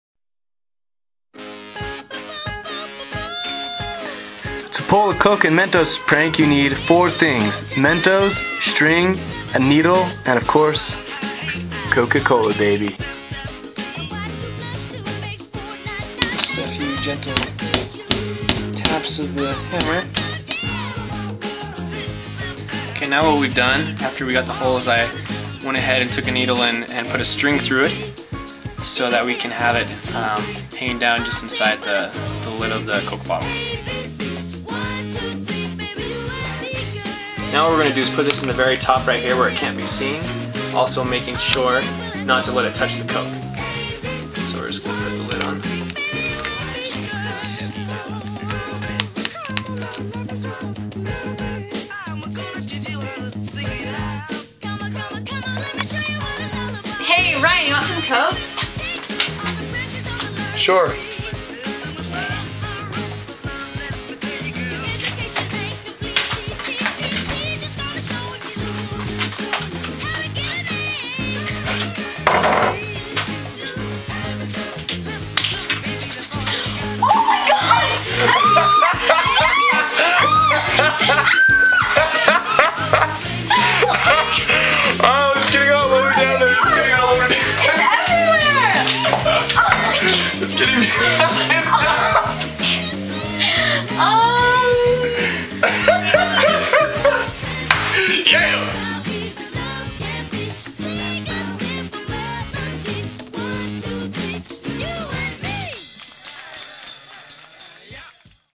How to Make a Coke Volcano
A simple how-to on how to play a good prank. I made this for my English class in college. It's pretty cool to note that you can do this with any size bottle, or even different soda drinks (the more mentos, the better). We used one mento in this shot; I used two mentos earlier in a practice and I was cleaning it off the ceiling.